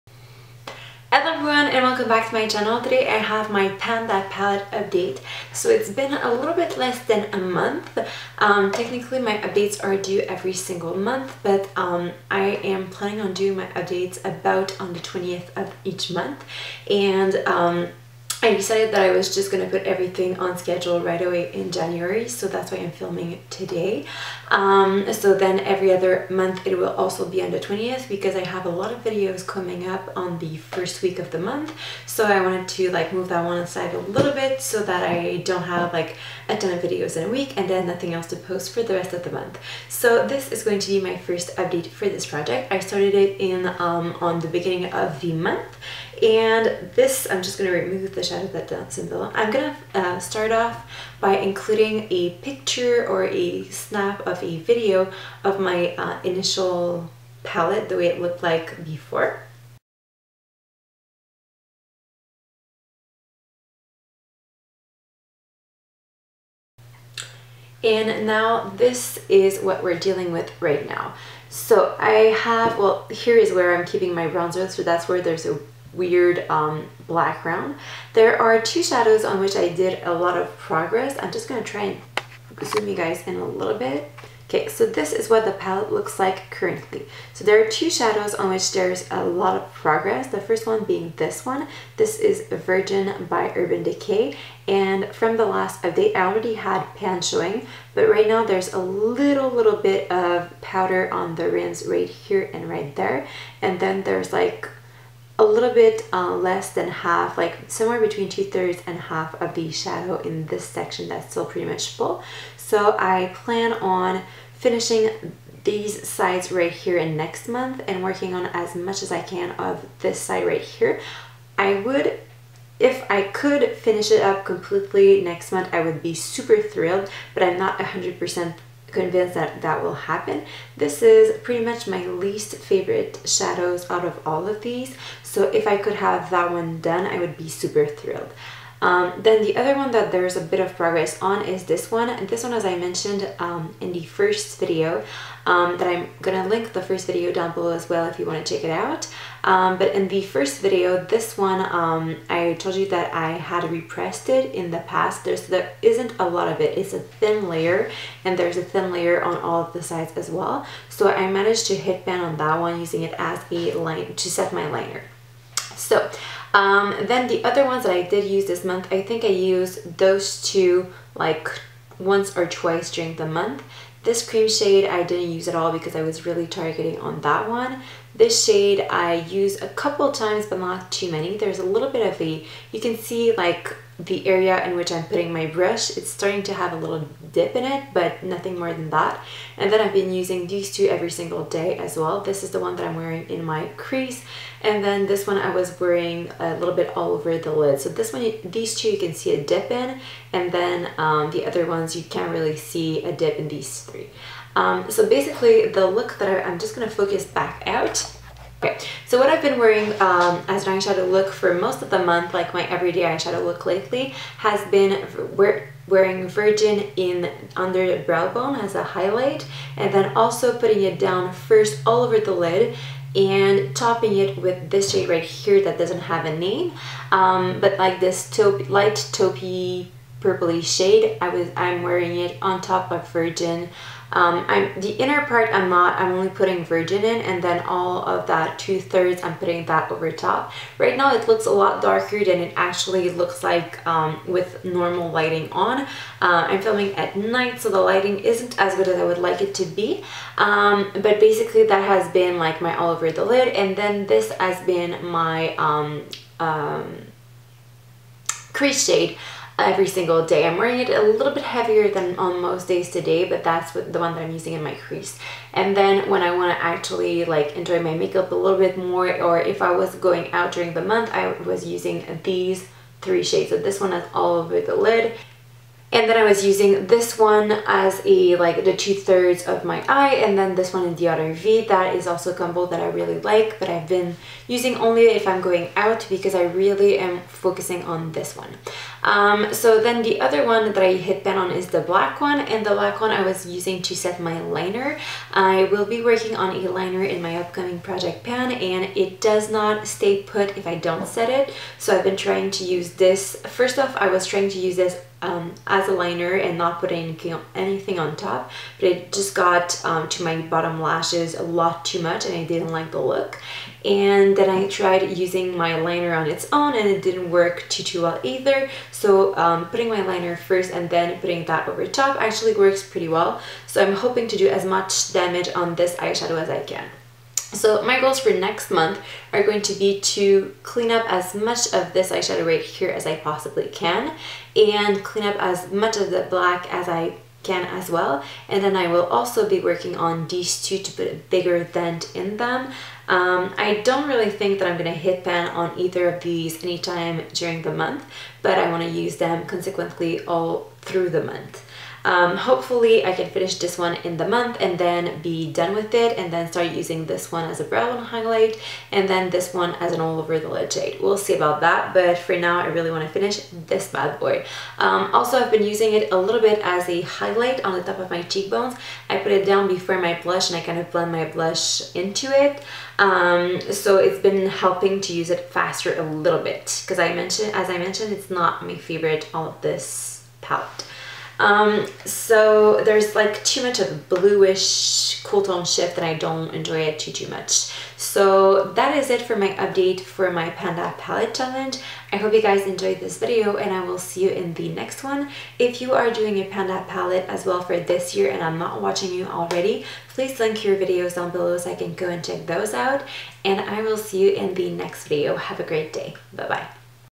Pan that palette - January update
Hey guys here is my almost one month update for my 2016 Pan that palette.

Here is the list of shadows mentioned :
Lise Watier les velours quad (matte brown)
Lise Watier les velours quad (taupe)
Lise Watier les velours quad ( matte black )
Urban Decay Virgin (naked 1)
Urban Decay Dope (Vice 2)
Urban Decay Strange (naked 3)
Urban Decay Factory (naked 3)
Urban Decay Liar (naked 3)